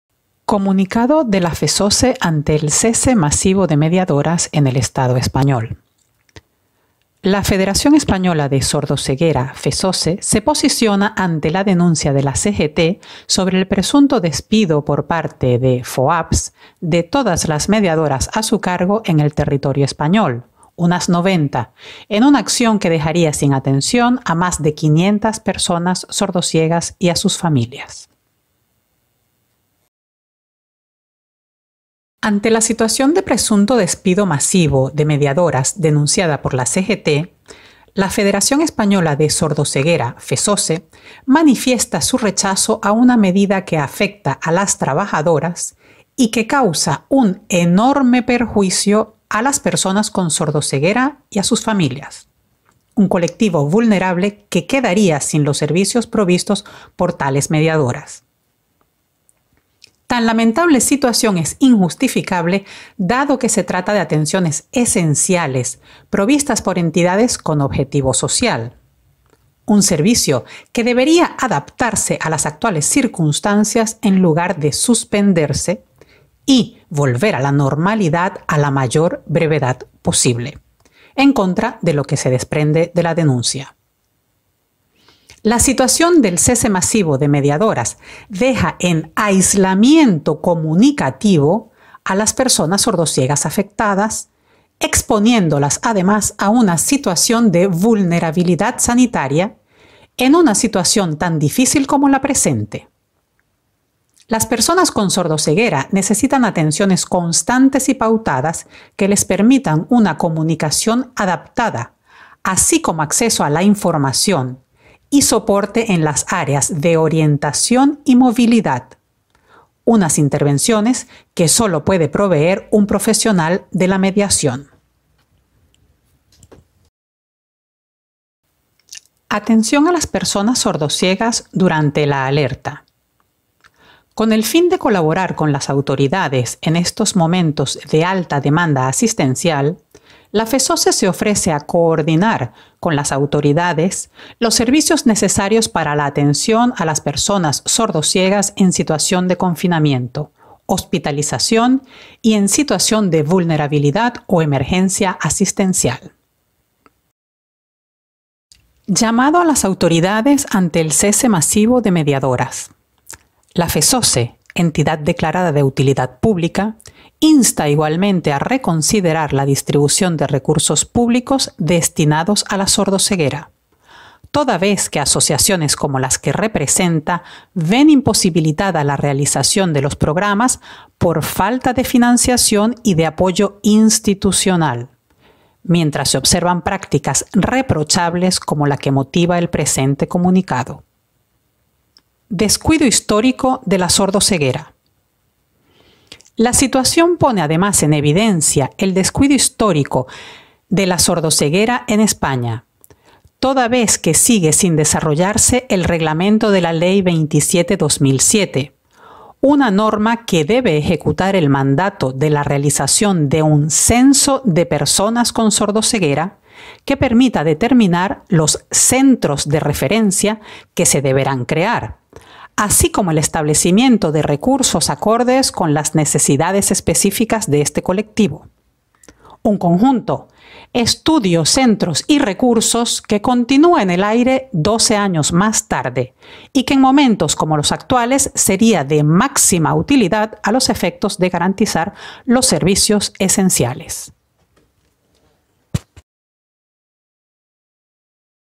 Comunicado de la FESOCE ante el cese masivo de mediadoras en el estado español
La Federación Española de Sordoceguera (FESOCE) se posiciona ante la denuncia de la CGT sobre el presunto despido por parte de FOAPS de todas las mediadoras a su cargo en el territorio español, unas 90, en una acción que dejaría sin atención a más de 500 personas sordociegas y a sus familias